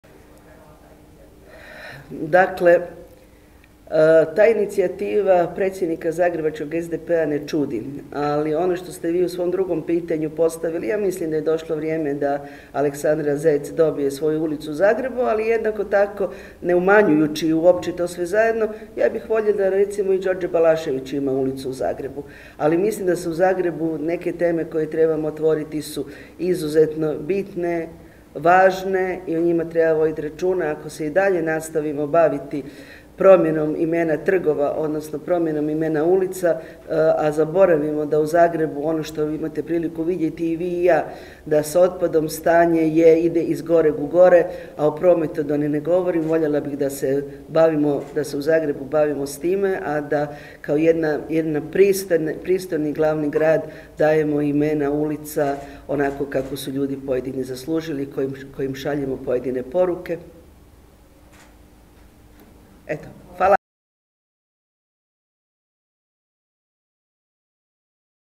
Anka Mrak Taritaš: Voljela bih da Balašević dobije ulicu u Zagrebu
Na konferenciji za medije održanoj u Saboru zastupnica Anka Mrak Taritaš govorila je o temi "Žene i ženska prava u državnom proračunu."